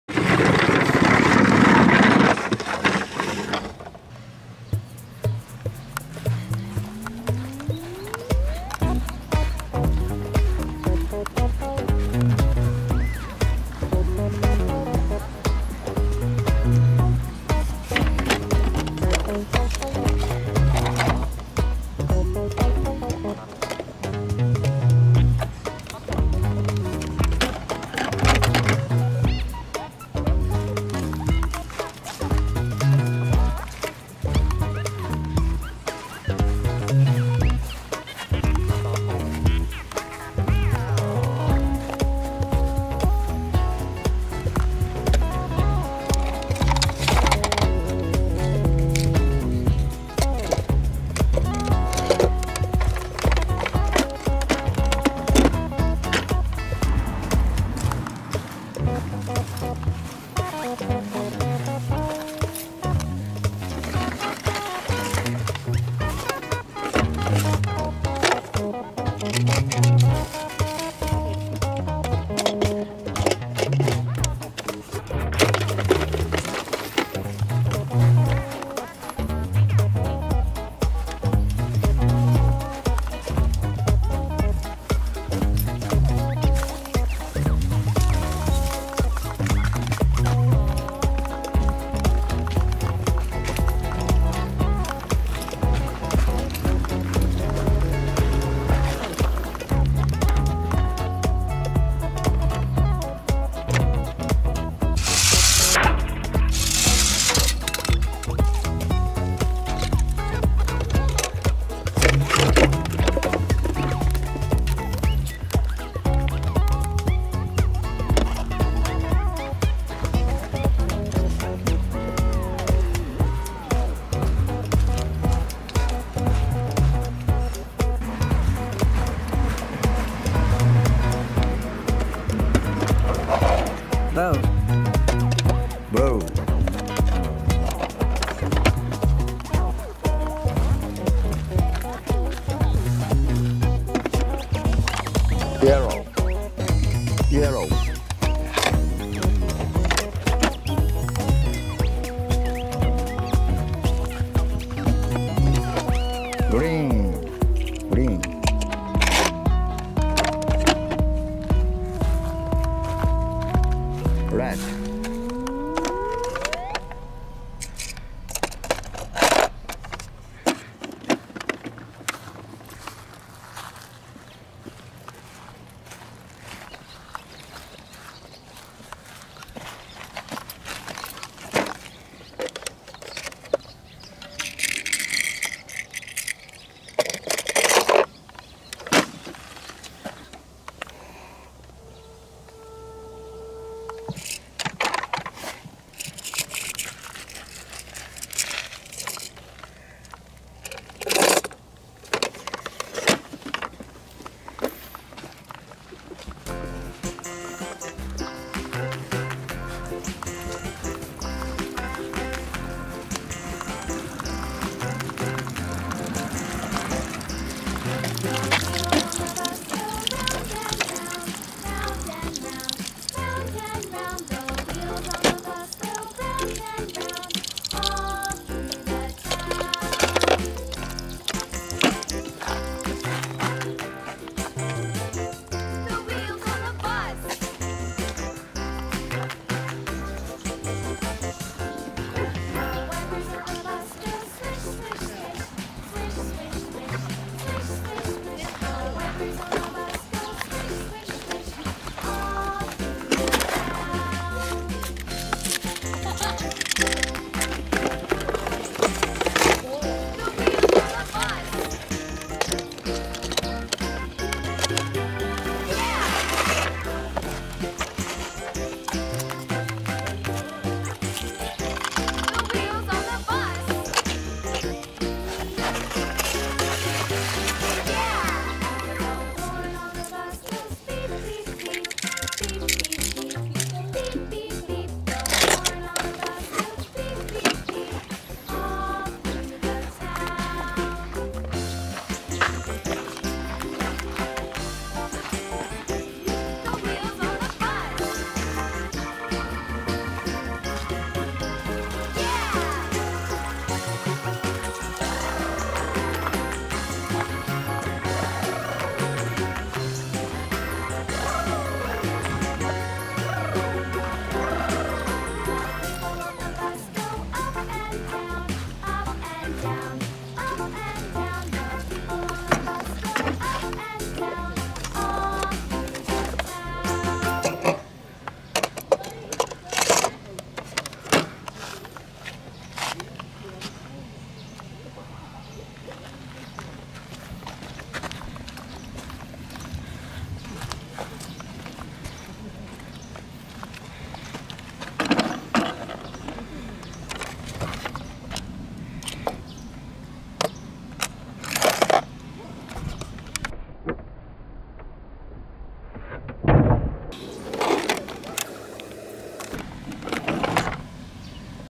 Chuggington Wilson Carry Case with Marvel, Thomas, Tayo Hide and Seek!
This big Wilson case can hold 17 trains!
Find hidden friends in the park and store them in a carry case!